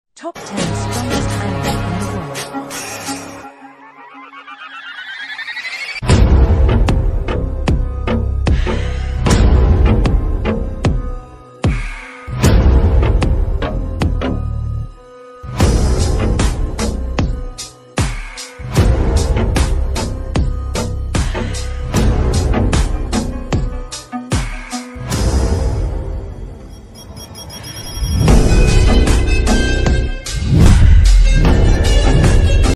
TOP 10 strongest animal in the world
🦁💪 Check out the top 10 strongest animals on Earth in our latest YouTube short! From the powerful African elephant to the mighty great white shark, witness nature's true titans in action. Don't miss out – watch now and prepare to be amazed!